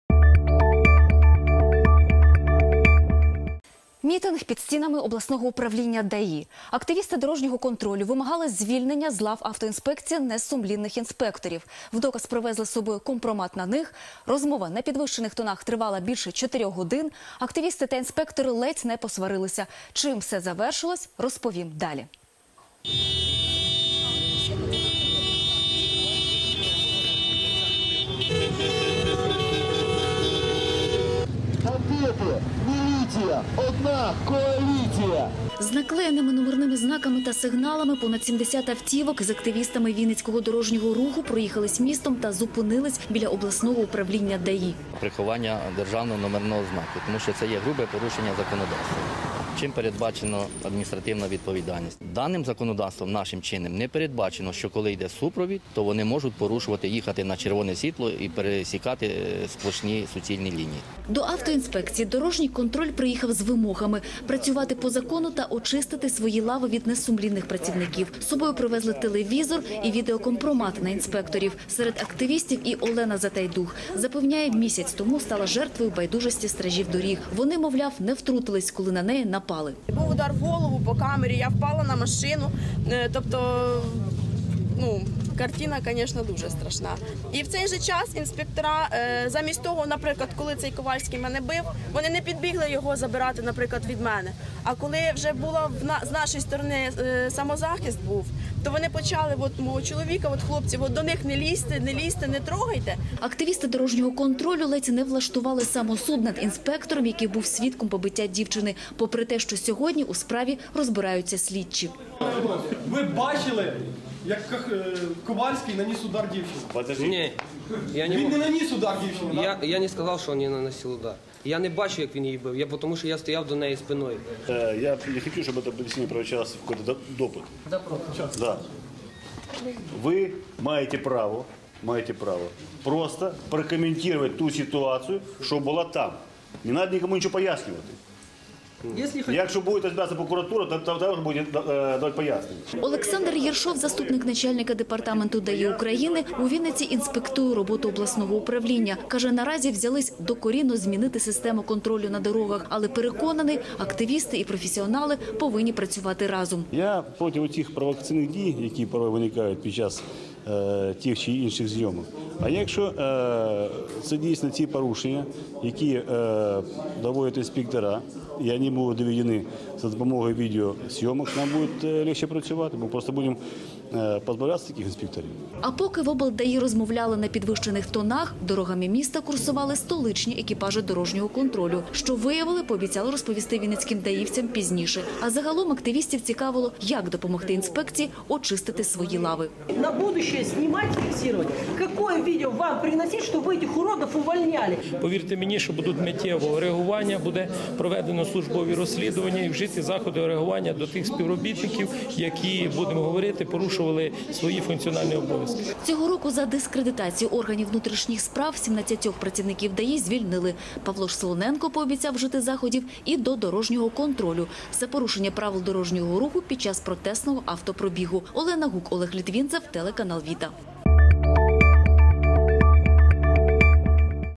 Телеканал ВІТА Новини 2014-09-29 Чому мітингували під обласною автоінспекцією
Мітинг під стінами обласного управління ДАІ.  Активісти дорожнього контролю вимагали звільнення з лав автоінспекції несумлінних інспекторів. В доказ привезли з собою компромат на них. Розмова на підвищених тонах тривала більше чотирьох  годин.